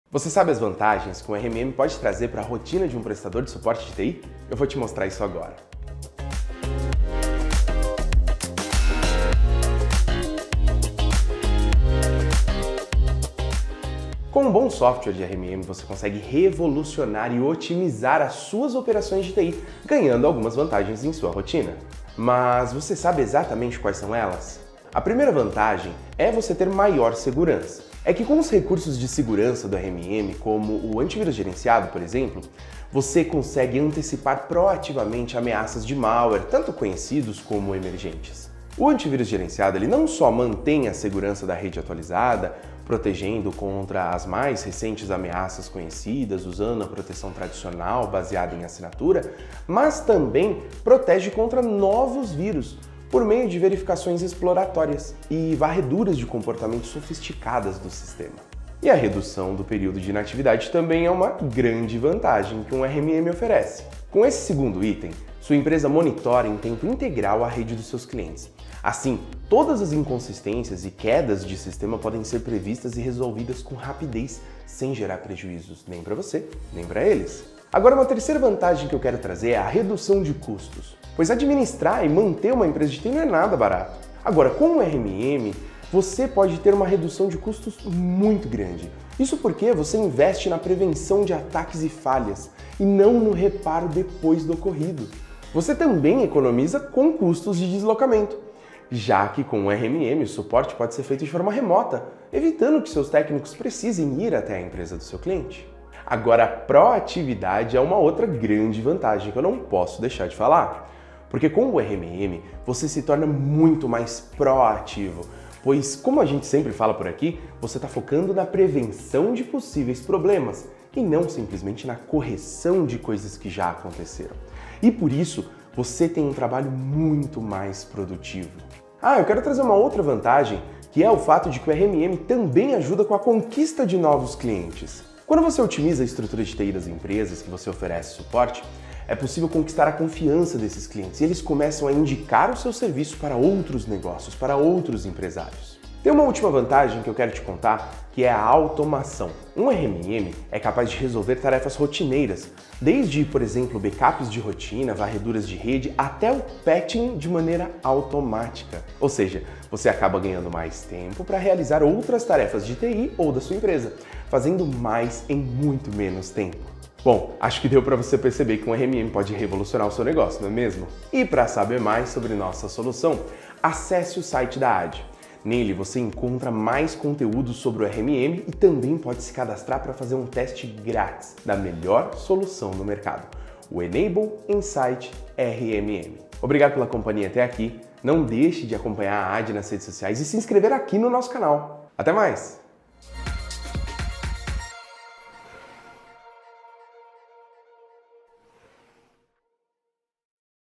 Vantagens de utilizar um RMM na sua empresa de TI
Quer saber quais as vantagens em utilizar um RMM? Nesse vídeo você vai descobrir como ele pode transformar o atendimento que você oferece para os seus clientes!

Não perca tempo e confira agora mesmo!